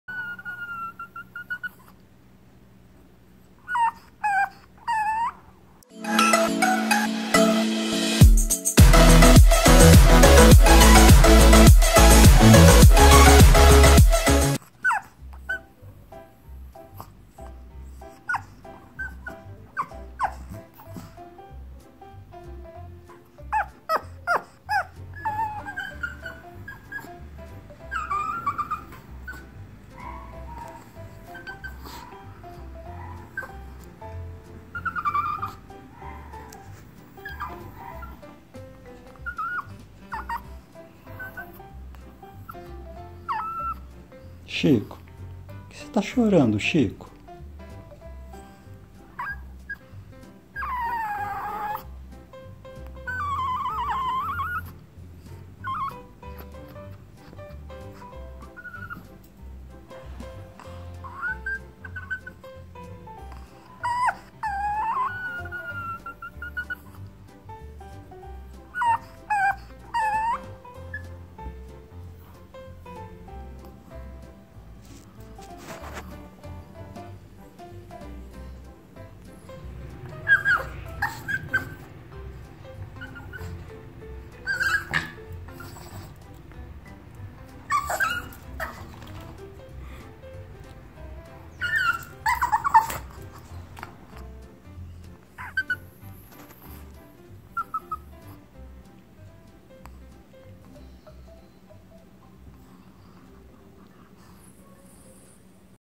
Chico o Chihuahua em "Cachorro Chorando!!!" Crying Chihuahua!!!
O Chico meu chihuahua, chora para tudo, ele é muito emotivo. Cachorro chorando o dia inteiro em casa...rsrs!!! Mas sei sempre qual o motivo... ir passear, um carinho ou um pouquinho de atenção mesmo. E ele sempre consegue o que quer com sua tática...rsrs!!!